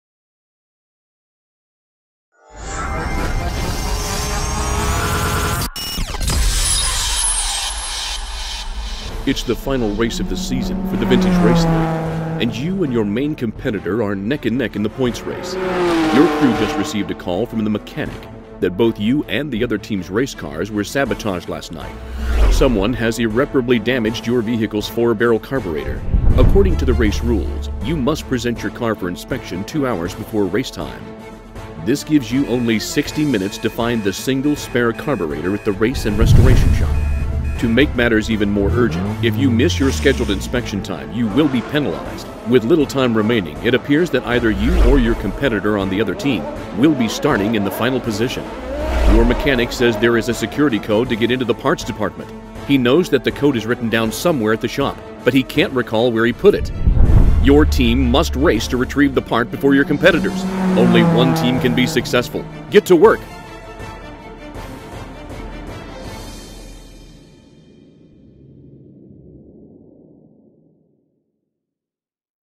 The Race Room at The Escape Room Fishers
It’s the final race of the season for the Vintage Race League and you and your main competitor are neck and neck in the points race. Your crew just received a call from the mechanic that both you and the other team’s racecars were sabotaged last night. Someone has irreparably damaged the four-barrel carburetor. According to the race rules you must present your car for inspection exactly two hours before race time. The deadline will give you only 60 minutes to find the spare carburetor at the Race and Restoration Shop.